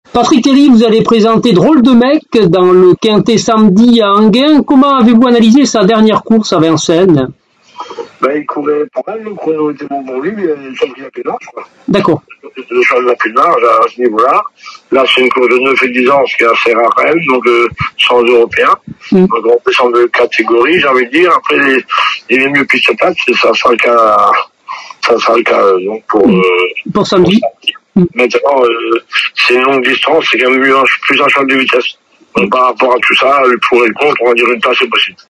Patrick Terry, entraîneur de Drôle de Mec (16/07 à Enghien)
Patrick Terry, entraîneur de Drôle de Mec (8) nous parle de ses chances dans le Quinté+, le Prix de la Porte de Sèvres sur l’hippodrome d'Enghien le 16/07/2022.